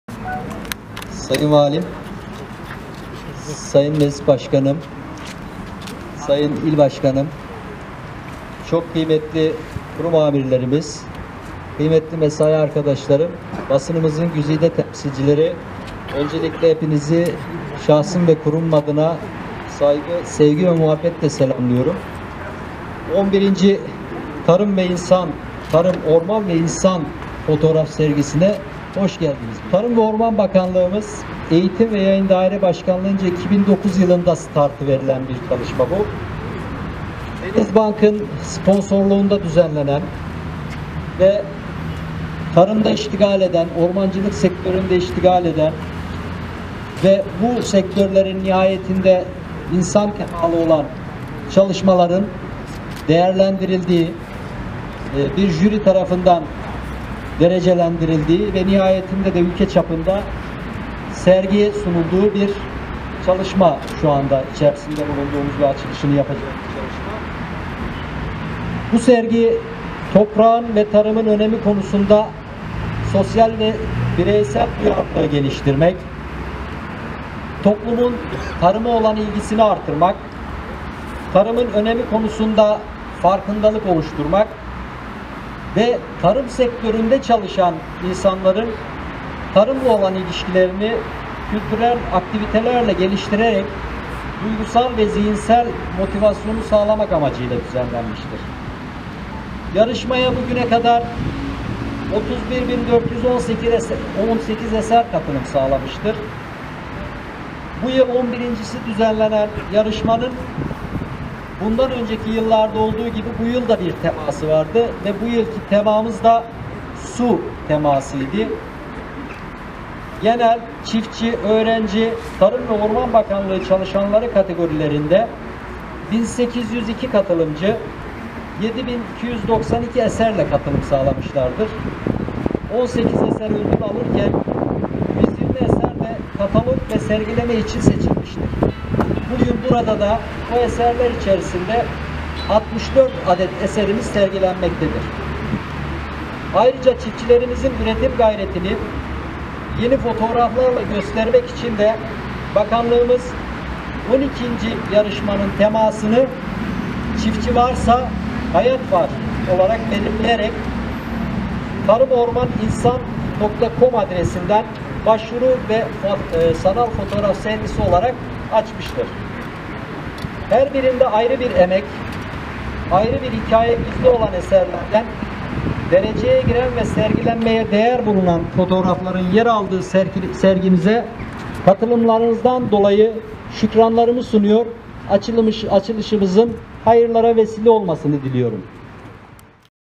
11 TARIM VE İNSAN FOTOĞRAF SERGİSİ AÇILDI
Tarım ve Orman Bakanlığı, Eğitim Yayım ve Yayınlar Dairesi Başkanlığı tarafından geçtiğimiz yıl düzenlenen 11 "TARIM VE İNSAN" ulusal fotoğraf yarışmasında dereceye giren ve sergilenmeye değer bulunan fotoğrafların görücüye çıktığı fotoğraf sergisi törenle açıldı.

Yalova İl Tarım ve Orman Müdürlüğü bahçesinde pandemi kurallarına özen gösterilerek yapılan sergi açılışına Yalova Valisi Muammer Erol, Vali Yardımcısı Mehmet Bahattin Atçı, İl Müdürü Suat Parıldar, İl Genel Meclisi Başkanı Hasan Soygüzel, Kurum ve Kuruluş Müdürleri, Şube ve İlçe Müdürleri, Kooperatif ve Birlik Başkanları, Teknik Personeller ve
fotoğraf severler katıldı.

İl Müdürü Suat Parıldar açılış öncesi yaptığı konuşmada " Tarım ve Orman Bakanlığımız, Eğitim ve Yayın Dairesi Başkanlığınca 2009 yılından itibaren Denizbank sponsorluğunda düzenlenen ve kamuoyunda yoğun ilgi gören Tarım Orman ve İnsan Fotoğraf Yarışmasında dereceye giren eserler tüm illerimizde sergilenmektedir.

Bu sergi; Toprağın ve Tarımın önemi konusunda sosyal ve bireysel duyarlılığı geliştirmek, toplumun tarıma ilgisini artırmak, tarımın önemi konusunda farkındalık oluşturmak, tarım sektöründe çalışanların tarımla olan ilişkilerini kültürel aktivitelerle geliştirerek duygusal ve
zihinsel motivasyonunu sağlamak amacıyla gerçekleştirilmektedir.
Yarışmaya bugüne kadar toplam 31 bin 418 eser ile katılım sağlandı. Geçtiğimiz yıl 11’ incisi düzenlenen yarışmanın bu yılki teması “SU” oldu. Genel, Çiftçi, Öğrenci, Tarım ve Orman Bakanlığı çalışanları kategorinde 1802 katılımcı 7 bin 292 eserle katıldı. 18 eser ödül alırken, 120 eser de katalog ve sergileme için seçildi. Bugün burada 64 adet resim
sergilenmektedir.

Ayrıca çiftçilerimizin üretim gayretini yeni fotoğraflarla göstermeye devam eden Bakanlığımız, 12 yarışmanın temasını “ÇİFTÇİ VARSA HAYAT VAR" olarak belirleyerek adresinden başvuru ve sanal fotoğraf sergisini ziyarete sunmuştur.

Her birinde ayrı bir emek, ayrı bir hikâye gizli olan eserlerden dereceye giren ve sergilenmeye değer bulunan fotoğrafların yer aldığı sergimize katılımlarınız için teşekkür ederim" dedi

Suat Parıldar’ın konuşmasının ardından Vali Muammer Erol ve protokol üyeleri kurdele keserek serginin açılışını yaptılar. Suat Parıldar, Vali Muammer Erol ve beraberindekilere sergiyi gezdirerek sergilenen fotoğraflarla ilgili bilgiler verdi.